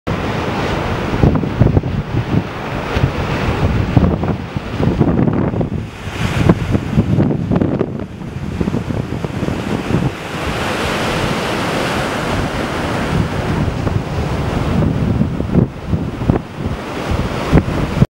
Big Wave Evening in the Harbor
After a Big Wave Day - the last of the waves roll pass the breakwall and into Redondo Beach Harbor.